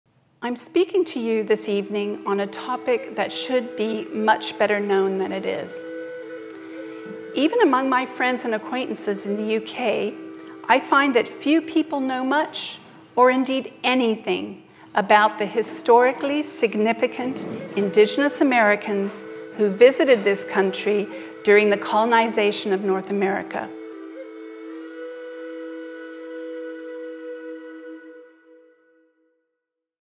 The Indigenous Americans that visited the UK during Colonisation
Taken from a Gresham lecture by Stephanie Pratt at Barnard's Inn Hall, London.

From 1600 – 1850, artists in England and, later, in North America depicted distinguished Native American tribal leaders, diplomats and warriors to commemorate their significance. Examples include Pocahontas (1617), and nineteenth century Lakota leaders Sitting Bull and Red Cloud.

For many years, these portraits were not properly understood, reducing many of the sitters to simple exemplars of the ‘noble savage’ cliché. In fact, these works are far more interesting once the sitters’ historical situation and agency are restored to them.